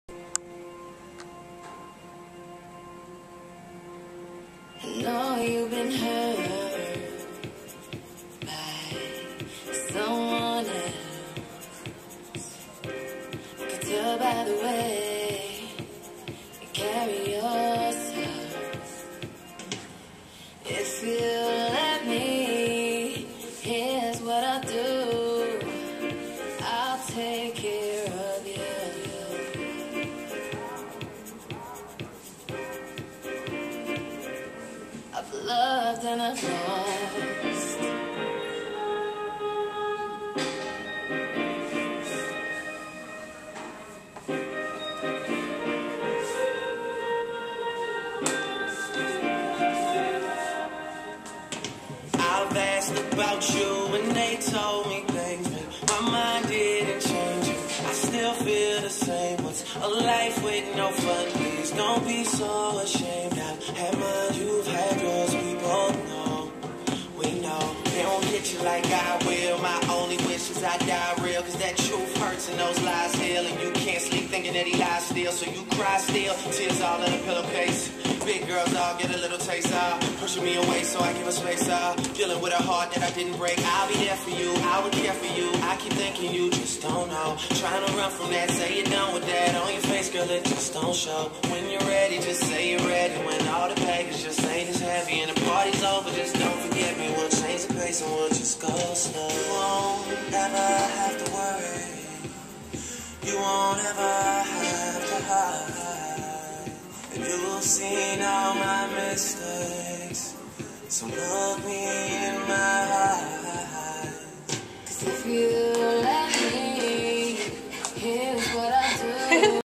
Sakura Le Improvised Dance
Sakura Le doing an improvised dance in a hallway